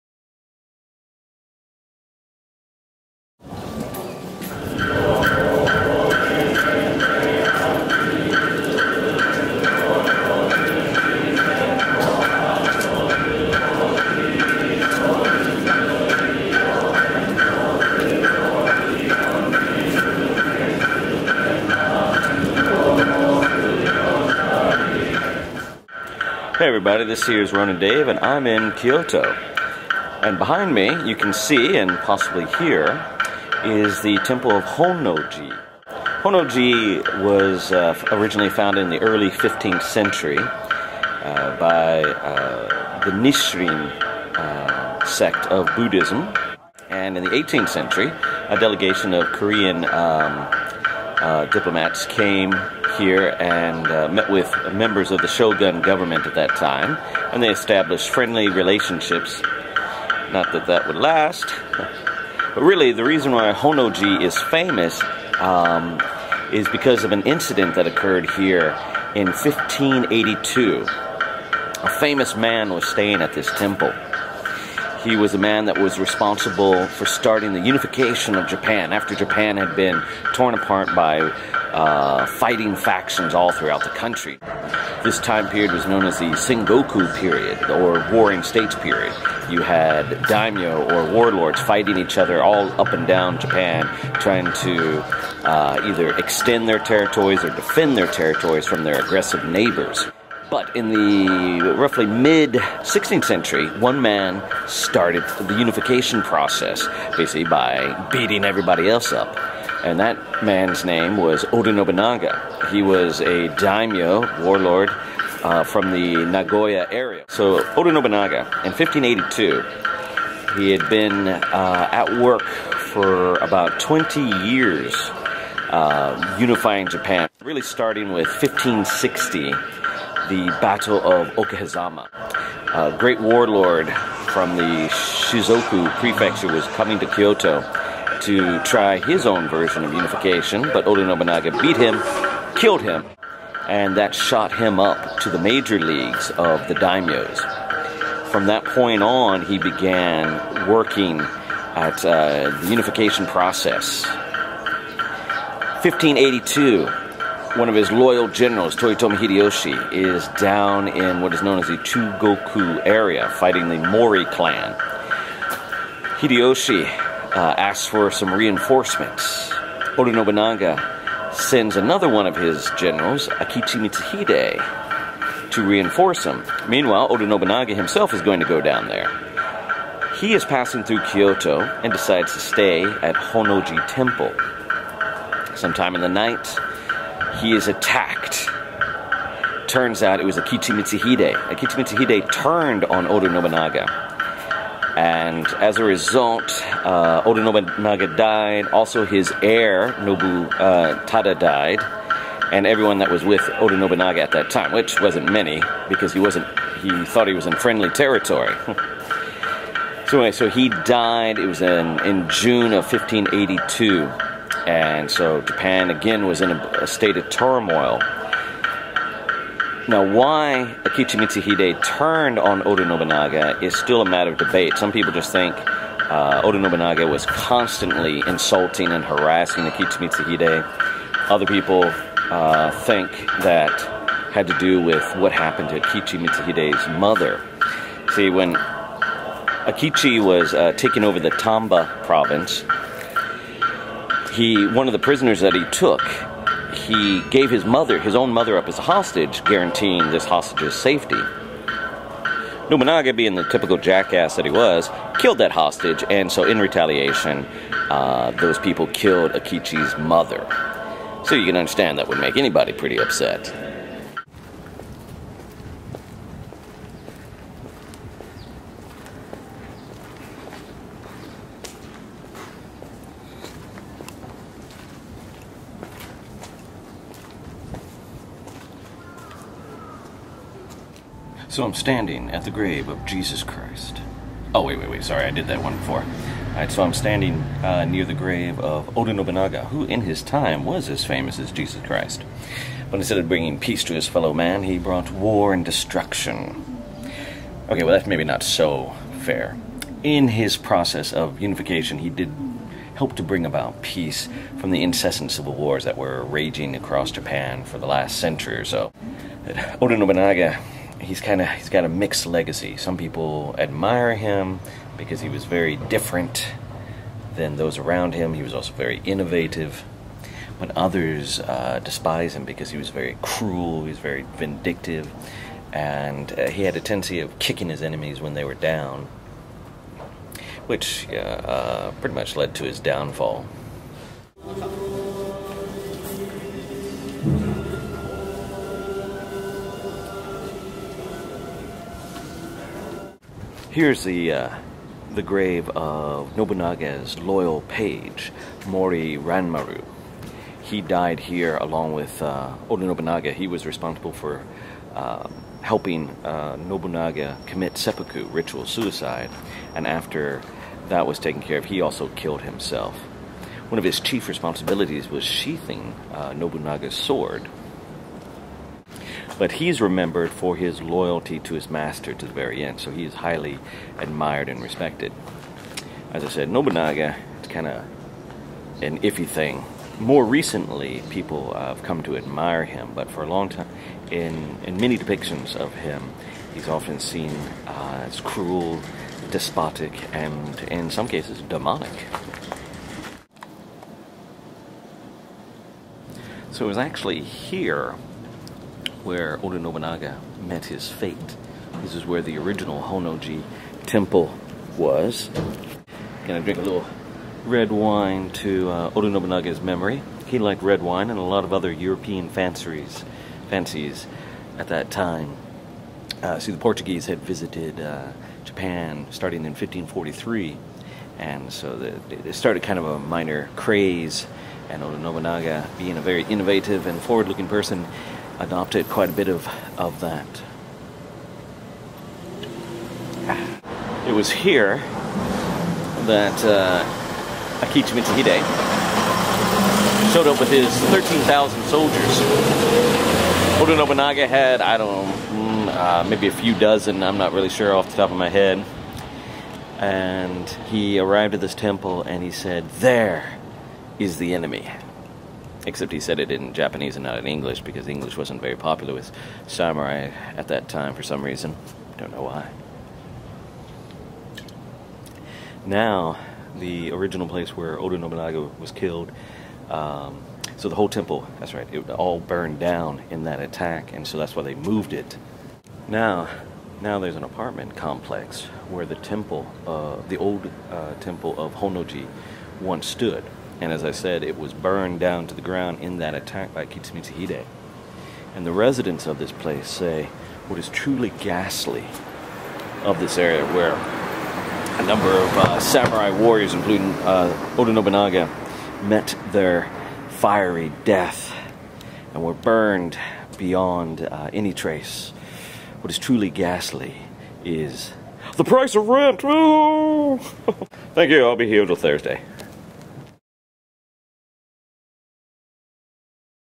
Japanese History - Honno-ji Temple and the Death of Oda Nobunaga
While in Kyoto recently, I payed a visit to the temple Honno-ji where the great 16th century warlord Oda Nobunaga met his fiery death.